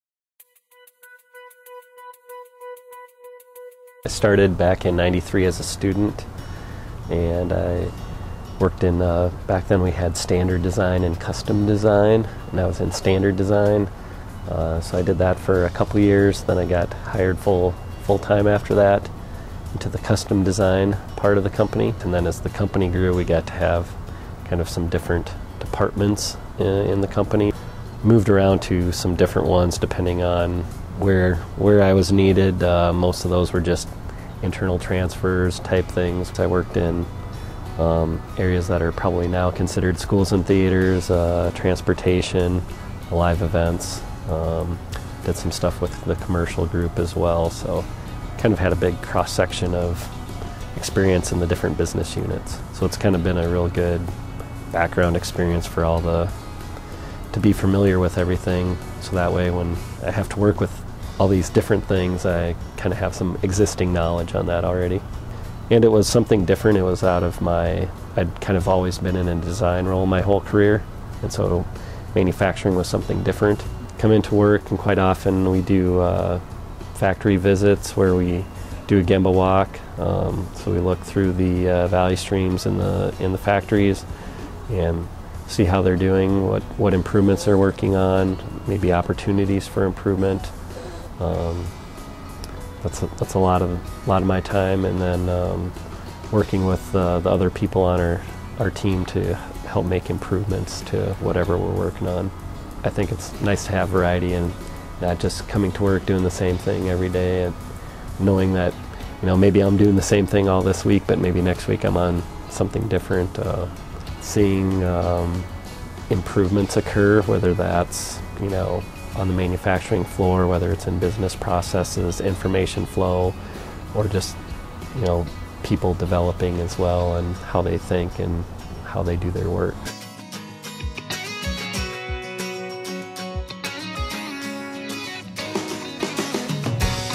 Day in the Life: Engineering Manager
A Daktronics employee discusses and demonstrates the role of Engineering Manager.